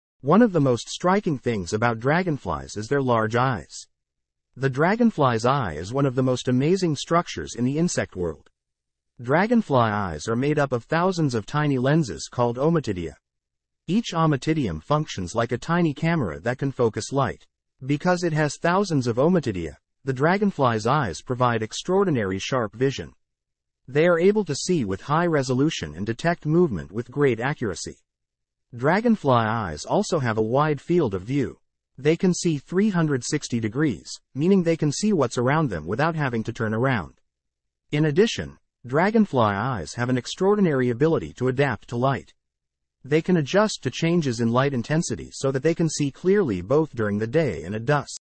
Dragonfly eyes like a 360 degree camera
The dragonfly's eye is one of the most amazing structures in the insect world. dragonflies are able to look around like a camera 360 degrees without having to turn their heads.
Find out the interesting facts about animals, plants, the solar system, natural phenomena and technological developments on this channel.